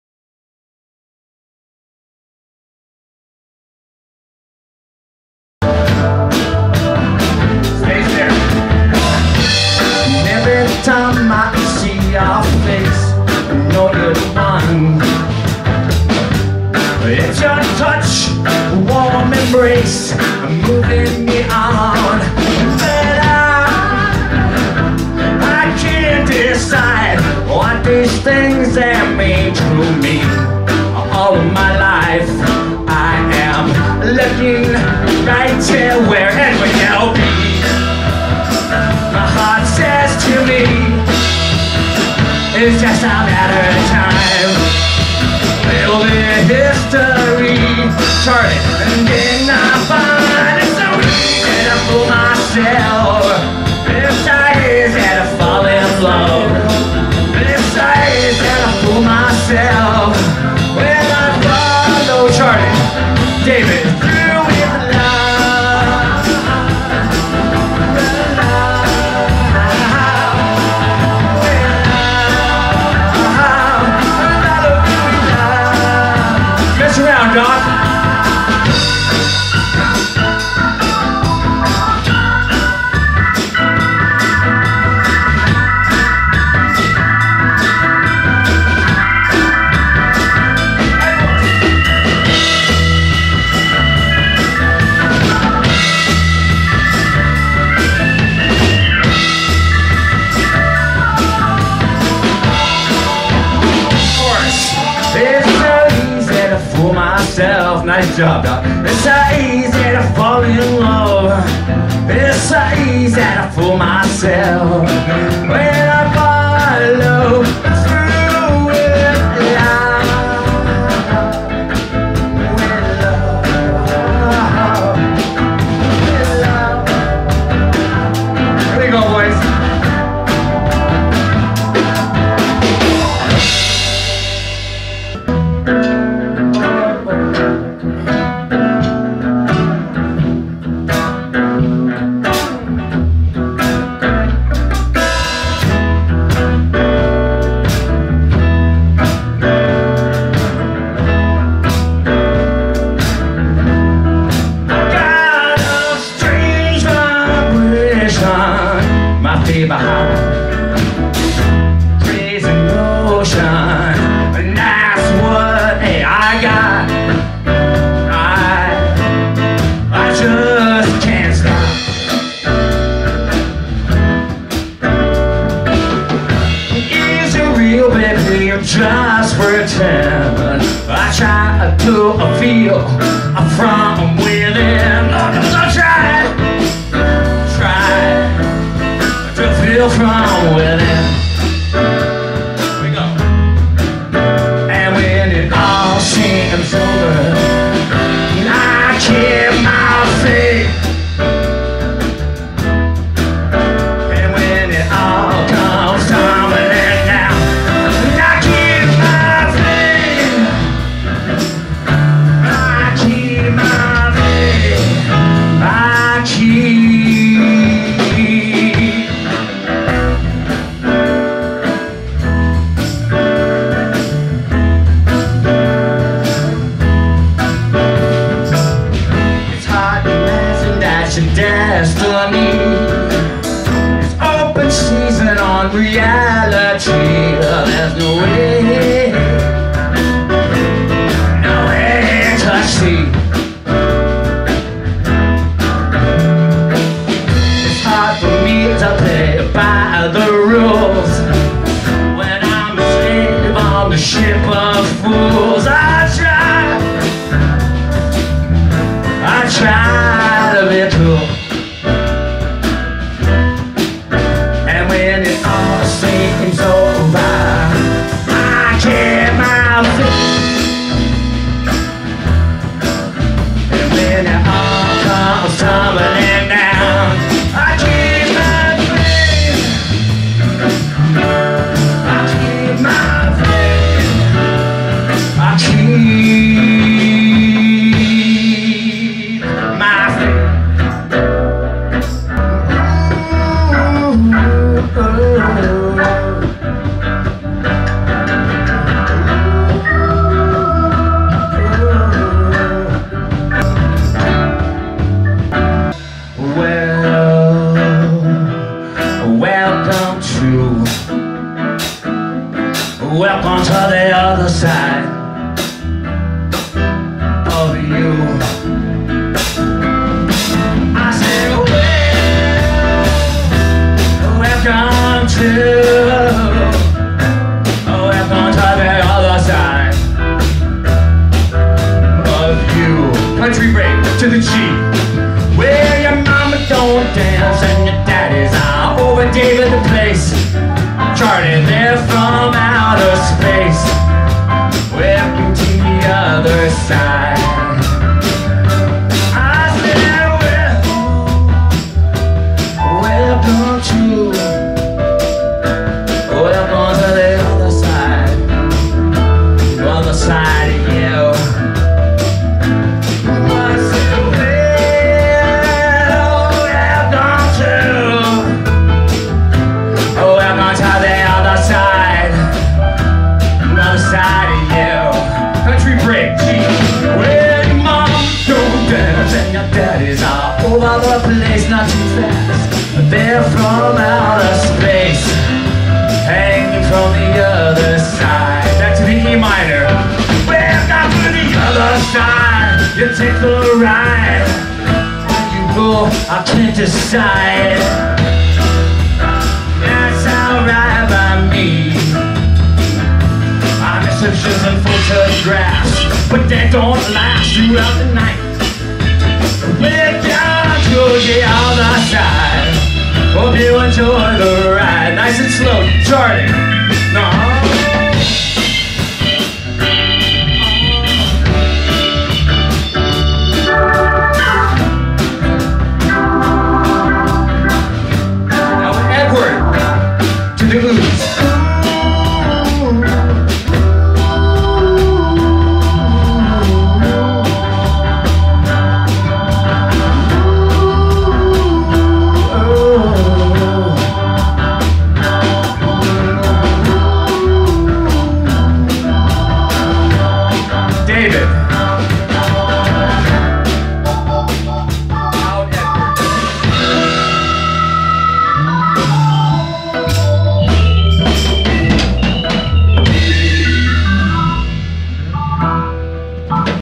The QuadMophonics
Recorded live at Blasmo
Kevin Mazur - Drums
Larry Howard Keyboard
L'angelo D' Silva - Bass
Rip Perkins Guitar